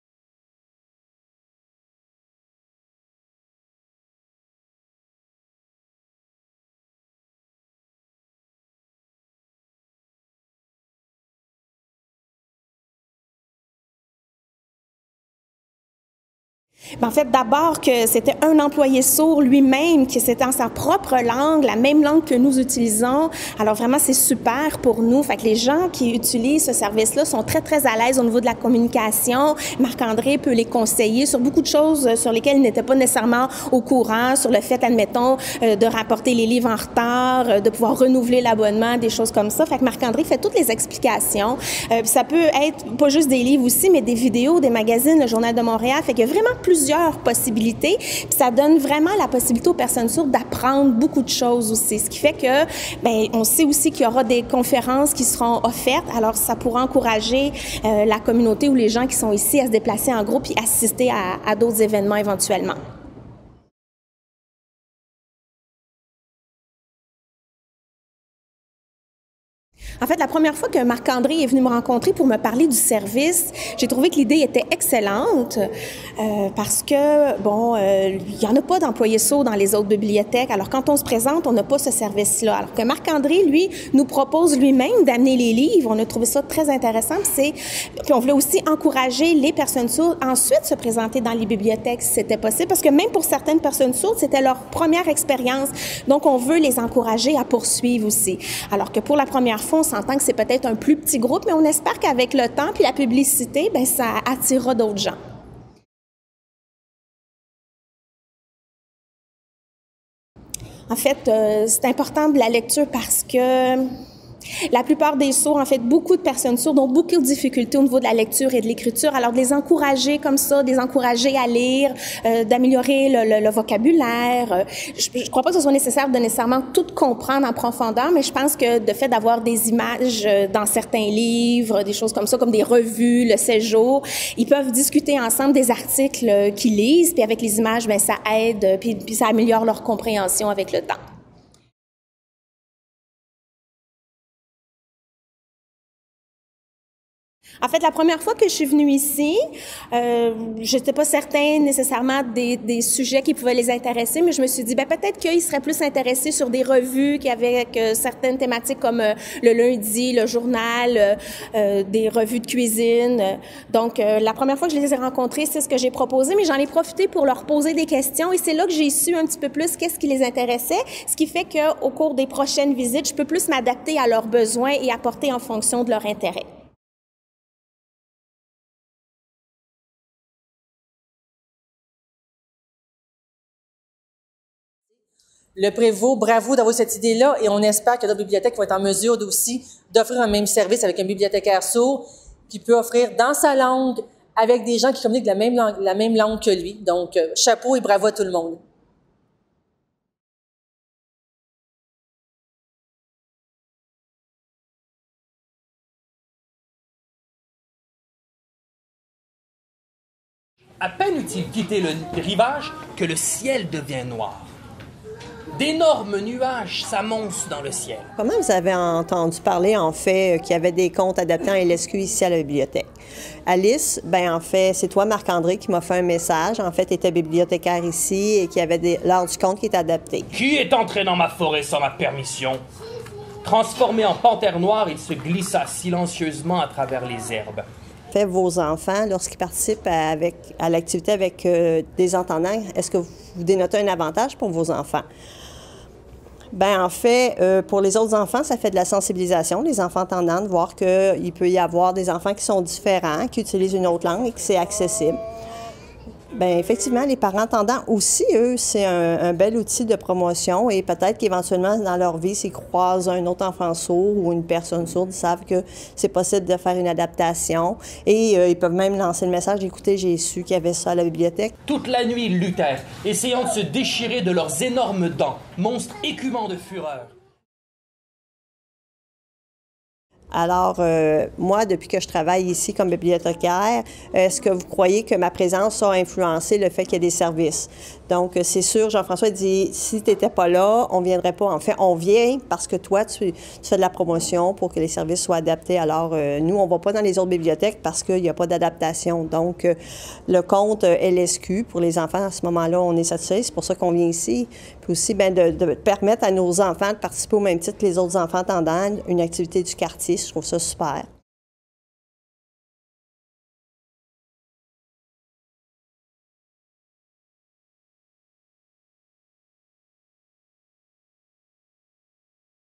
Mérite Ovation municipale 2015 : Arrondissement de Villeray–Saint-Michel–Parc-Extension
L'arrondissement a remporté le mérite Ovation municipale de l'Union des municipalités du Québec (UMQ), dans la catégorie Culture, patrimoine et interculturalité, pour le projet de la bibliothèque Le Prévost : Pour une bibliothèque adaptée aux besoins des personnes sourdes et malentendantes. Les municipalités lauréates ont été dévoilées lors de la Soirée gala, qui avait lieu le samedi 23 mai dernier au Palais des congrès de Montréal, dans le cadre des Assises annuelles de l'UMQ.